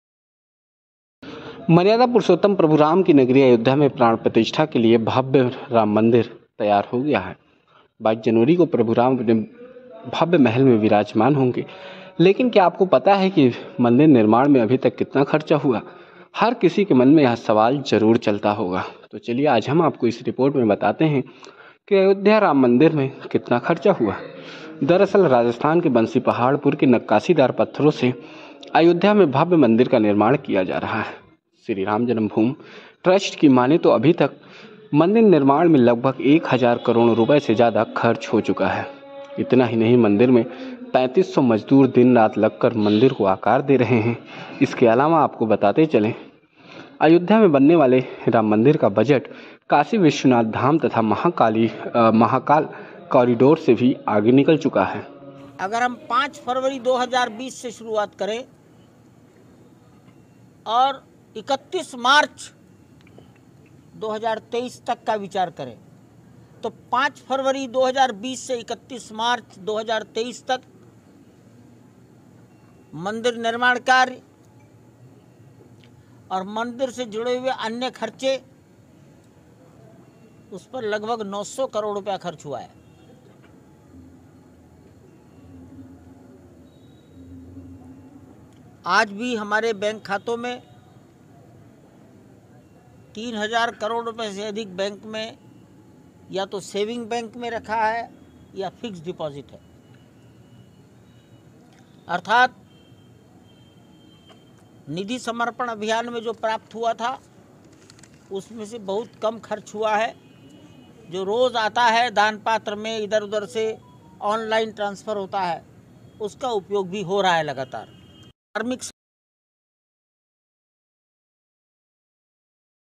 Kashi, महाकाल कॉरिडोर से आगे निकला Rma Mandir का बजट! जानें प्रथम चरण में कितना हुआ खर्च ?
Ram Mandir : अयोध्या में बनने वाले राम मंदिर का बजट काशी विश्वनाथ धाम तथा महाकाल कॉरिडोर से भी आगे निकल चुका है. गौरतलब है कि काशी विश्वनाथ धाम के सौंदर्यीकरण के लिए लगभग 900 करोड रुपए खर्च हुए थे जबकी महाकाल कॉरिडोर को विकसित करने के लिए करीब 850 करोड़ रुपए खर्च हुए थे.